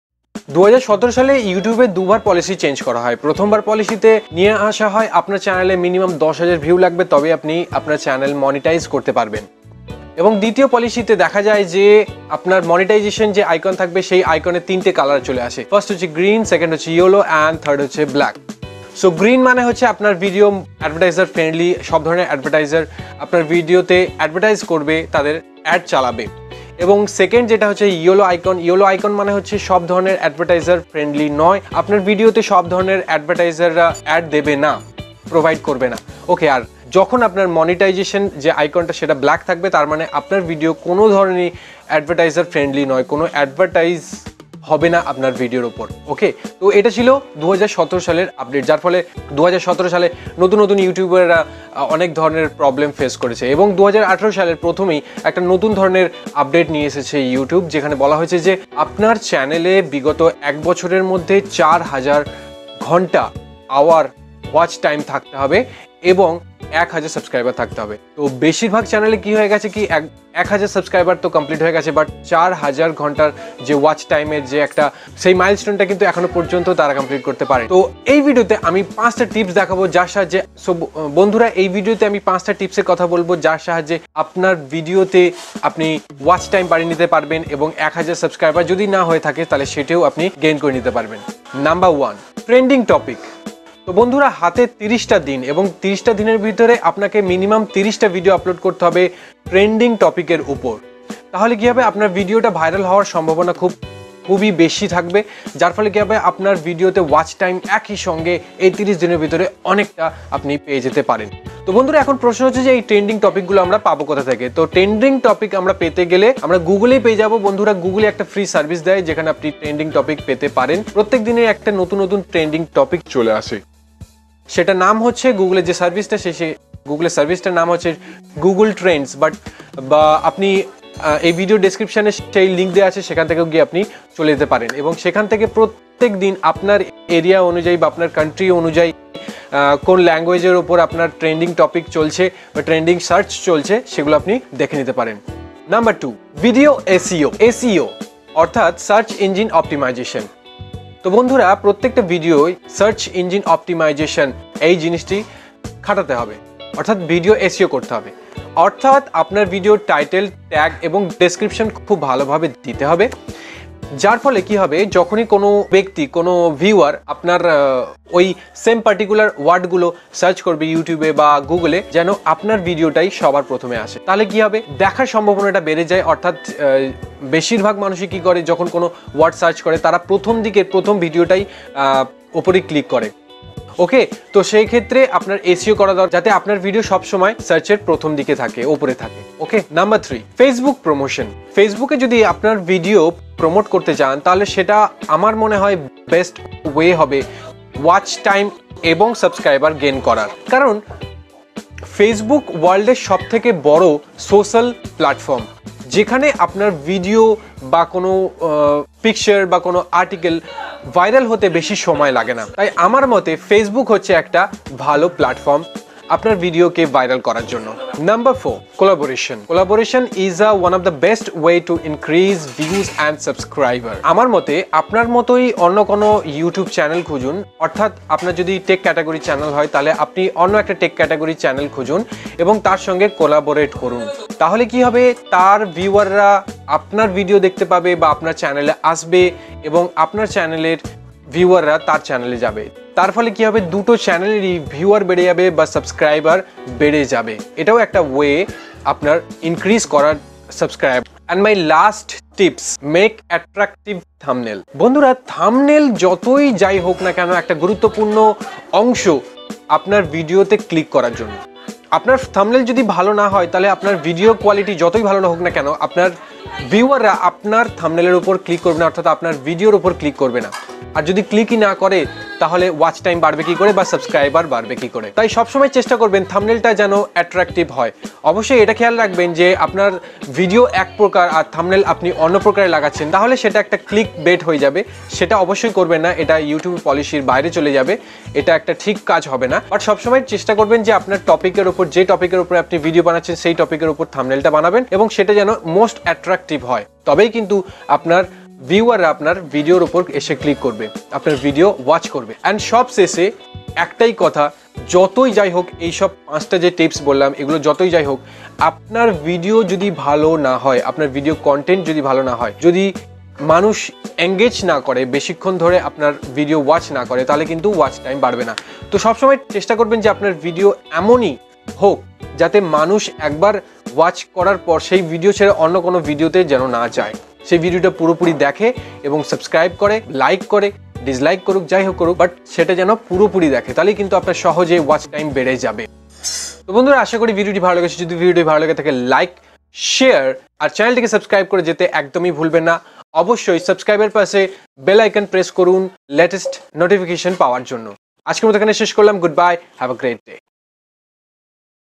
How To Get 4000 Hours WatchTime & 1000 SUBS In Easiest Way || Bangla tutorial
YouTube Change His Policy In 2018. If You Have A YouTube Channel And You Don't Have 4000 Hours Watch Time And 1K(1000) SUBSCRIBERS Then You Won't Monetize Your YouTube Channel And Videos. Here, In This Video I'm Gona Showing You How To Get 4000 Hour Watch Time And 1000 Subscriber In a month. It Is A Best Tips For New YouTubers.